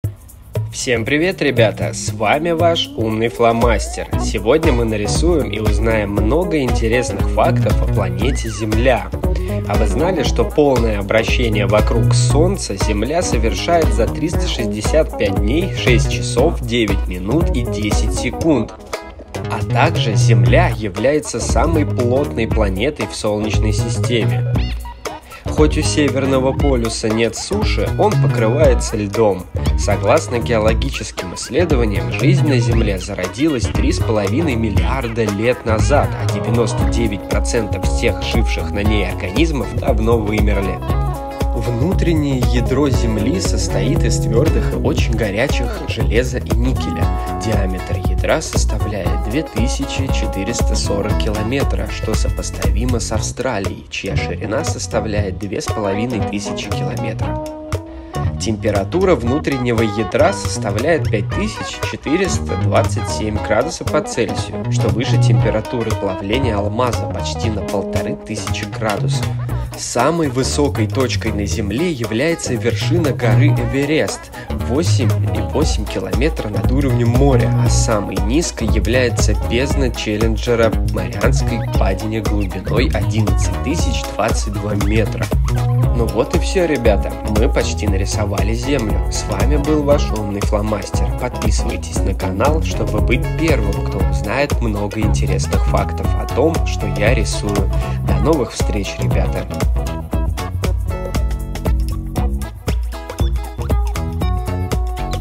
КАК НАРИСОВАТЬ ПЛАНЕТУ ЗЕМЛЯ-РИСУЕМ ЗЕМЛЮ HOW TO DRAW THE PLANET EARTH-DRAW THE EARTH
Всем привет! В этом видео мы увидим как правильно и красиво нарисовать планету Земля. А так же узнаем множество интересных фактов о нашей планете.

Hello everyone In this video, we will see how to draw the planet Earth correctly and beautifully. We will also learn a lot of interesting facts about our planet.
If you liked this video, then put a like)